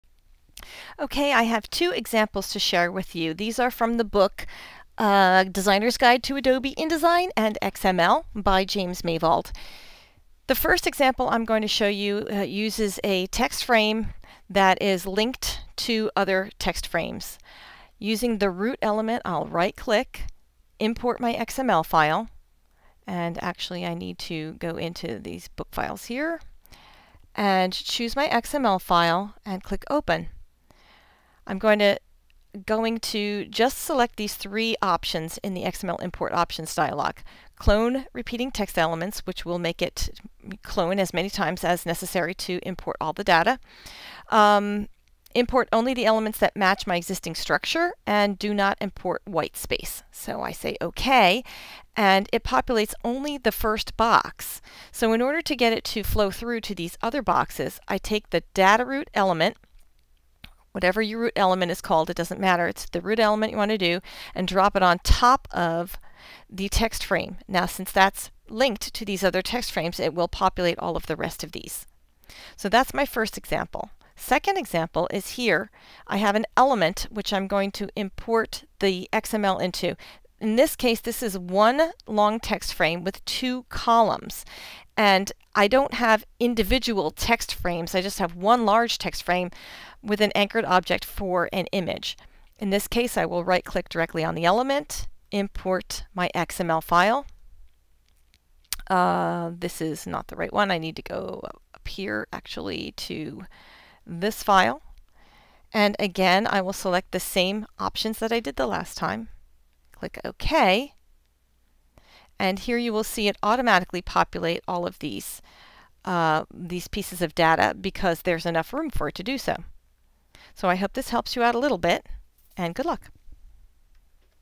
XML in InDesign CS4
A little sample showing different methods for importing XML into tagged placeholder text in InDesign CS4. Sample files are from the book "A Designer's Guide to Adobe InDesign and XML" by James Maivald.